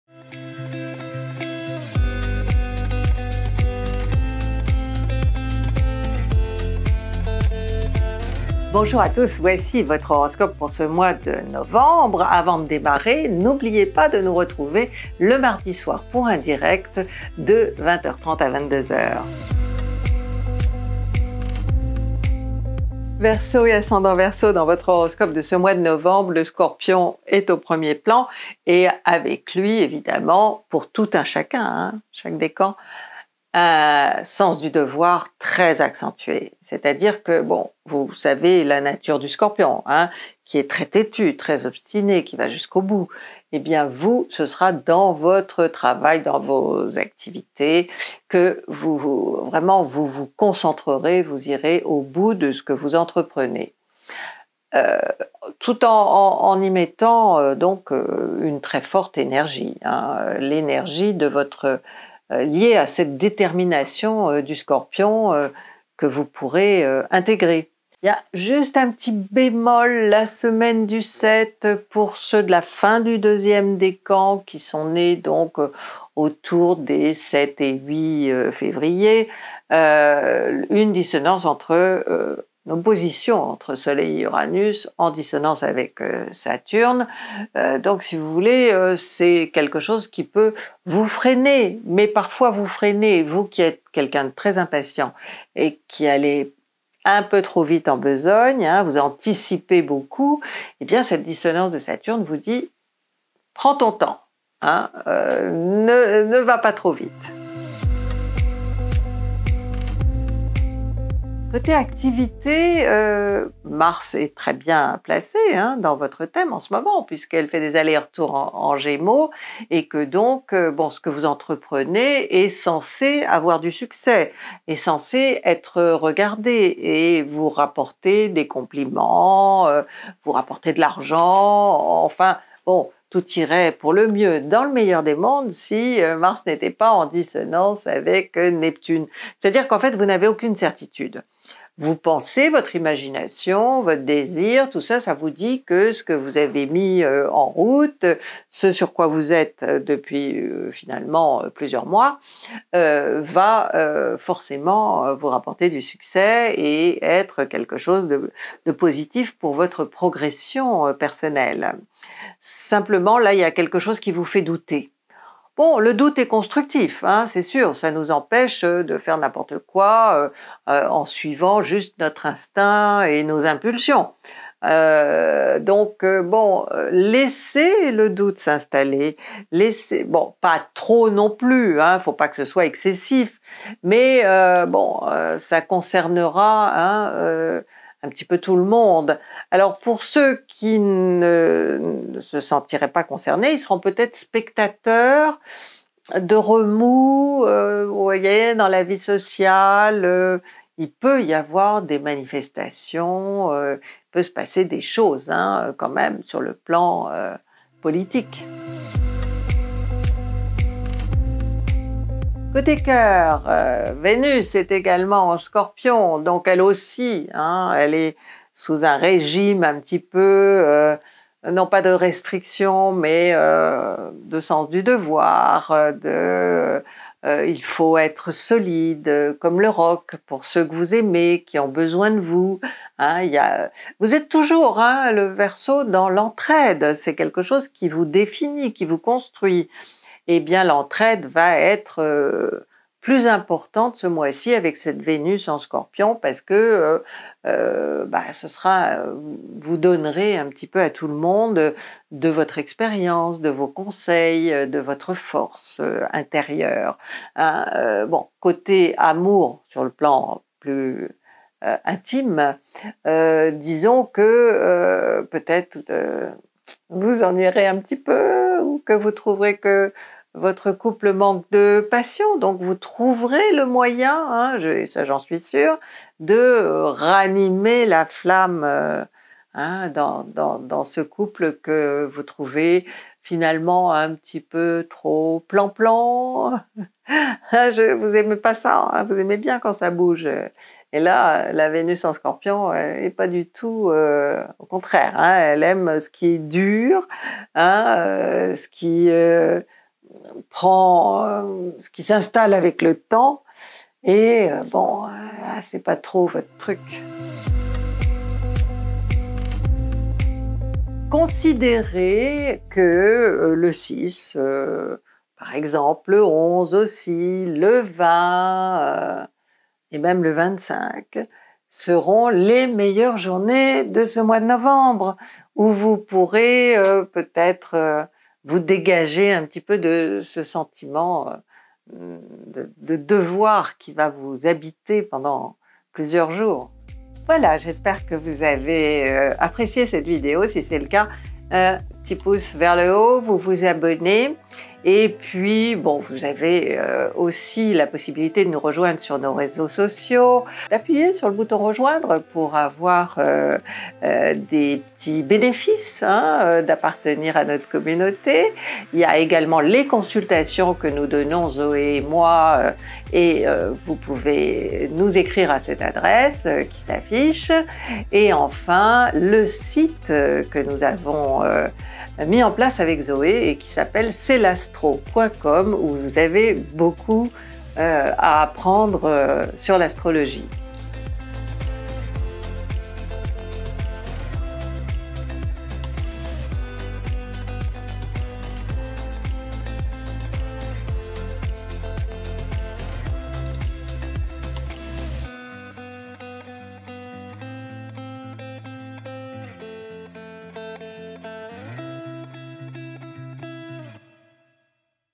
Horoscope Verseau ♒️ Novembre 2022 🕷 par Christine Haas
🌞 horoscope gratuit du signe du verseau pour le mois de novembre #2022 avec des conseils en astrologie et en psychologie.

⭐️ Retrouvez votre signe:
0:00 Introduction
0:19 Ambiance
2:13 Activités
3:54 Vie affective
5:33 Vos jours top
6:02 Bye bye